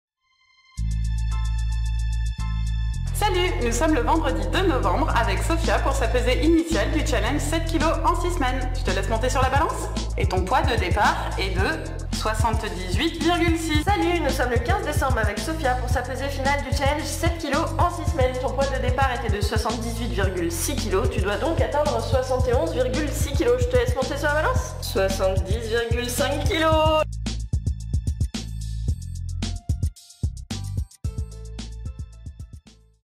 Transformation physique rapide Lyon
Sophia a perdu 8,100 kgs en 6 semaines à la Training Academy